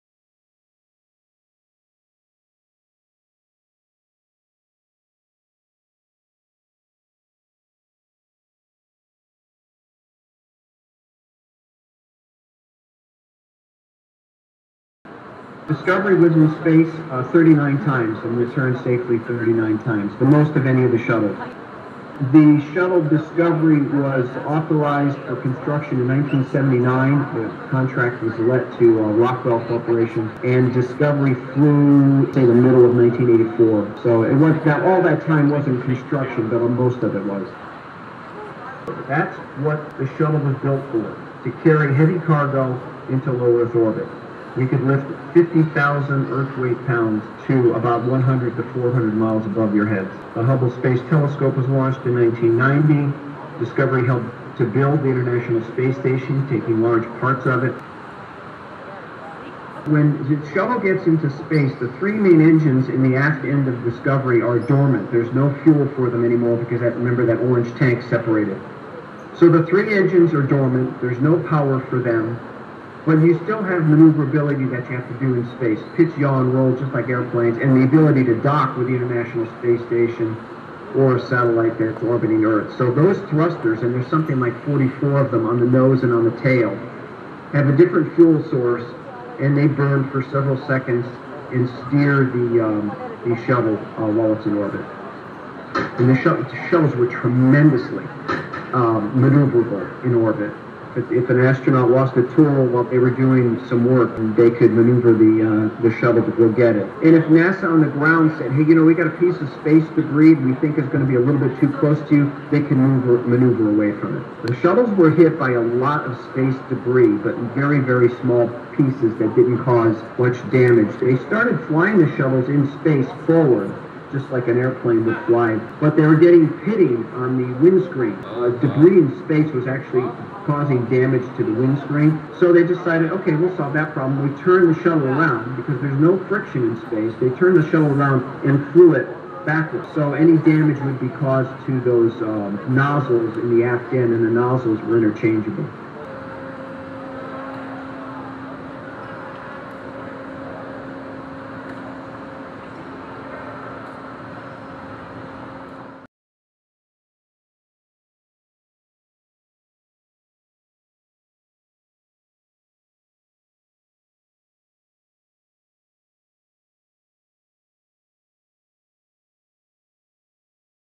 Space Shuttle Discovery History and Background 3D 180 VR
This 3D 180 VR experience takes a closer look at the Space Shuttle Discovery, which is currently on display at the Smithsonian National Air and Space Museum’s Steven F. Udvar-Hazy Center in Chantilly, Virginia. Discovery is one of the orbiters from the Space Shuttle program by the National Aeronautics and Space Administration (NASA), and it aggregated more spaceflights than any other spacecraft to date over its 27 years of service. During this video, a Smithsonian docent provides details about Discovery’s history and capabilities.

To learn more about the Space Shuttle Discovery at the Smithsonian, please visit the Smithsonian National Air and Space Museum

This video was shot with a Vuze XR camera, which you can learn more about

This video is best viewed with a VR headset, including the following:
Oculus Go,
Oculus Quest,
Oculus Quest 2,
Oculus Rift S,
HTC Vive,